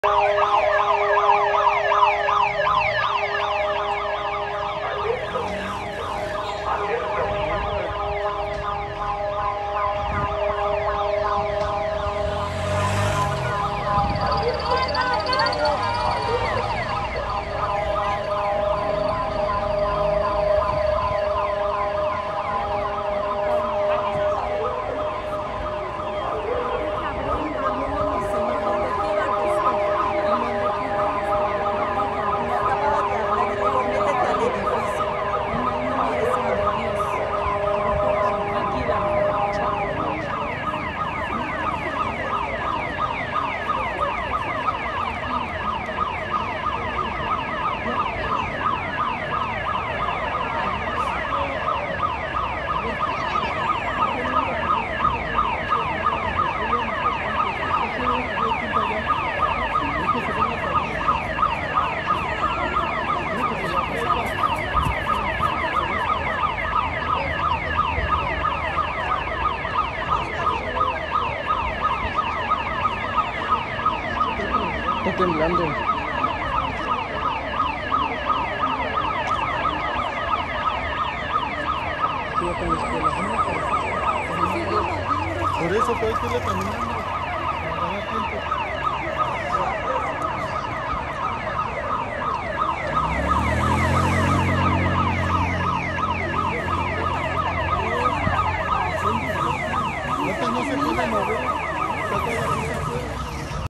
19 De Septiembre 2022 Nos Vuelve A Sorprender | Magnitud 7.4 | Cdmx 😥
Despues Del Silmulacro Vino El Real Se Sintio Fuerte En La Ciudad De Mexico Esto Paso En La Ciudad De Mexico 19 De Septiembre 2022
Saludos A Todos Esperando Todos Este  Bien Magnitud 7.4
SISMO Magnitud 7.4 Loc  63 km al SUR de COALCOMAN, MICH 19/09/22